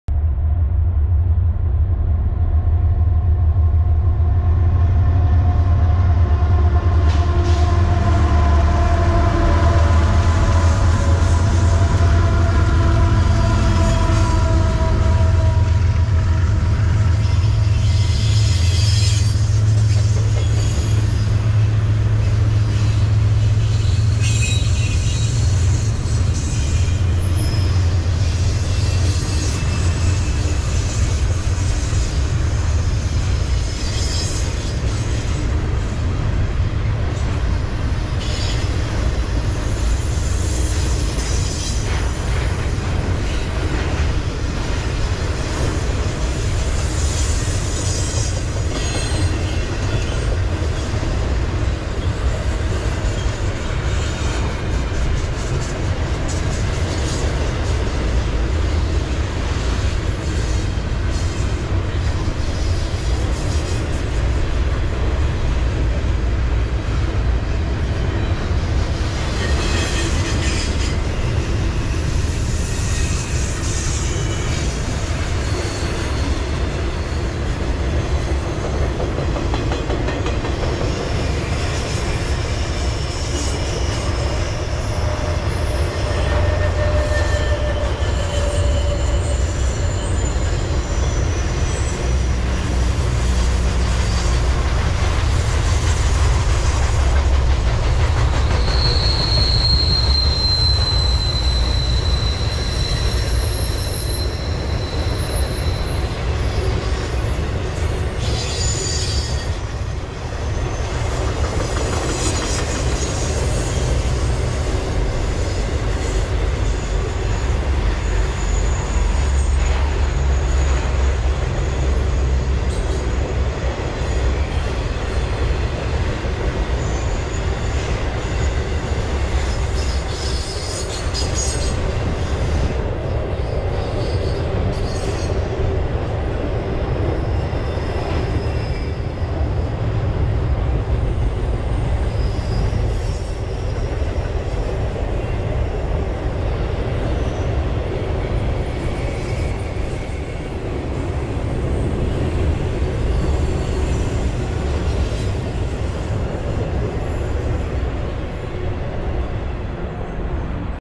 Westbound BNSF 4261/8091/5306/6021, Intermodal , "Red Rock Sub" Oklahoma City, OK, 11-01-21, GoPro
BNSF 6505
BNSF 8091
BNSF 5306
BNSF 6021

This is the Red Rock Subdivision that goes through Oklahoma City, OK. This is one of my favorite spots with the short time I was there. Its at the VRF (Virtual Rail Fan) camera location, which is located at the Oklahoma Model Railroad Association. This is a great location to watch trains and relax, very peaceful and secluded, lots of trains, and lots of great people who run the model club. Some days are busier than others, if u watch the VRF Oklahoma City CAM on youtube you can see what coming before it even arrives, wish I had more time here